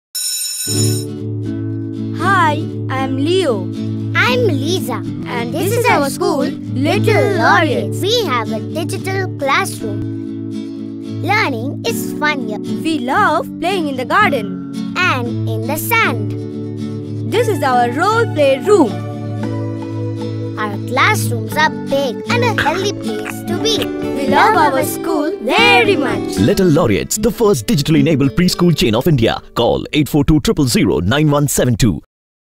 The Best Digital Enabled Pre-school Chain in Eastern India - Little Laureates
Meet Leo and Liza. They are sharing the reasons why they love their school Little Laureates so much!
Let your kids enjoy their school life like Leo and Liza. Join the first Digitally Enabled Preschool in Eastern India, Little Laureates.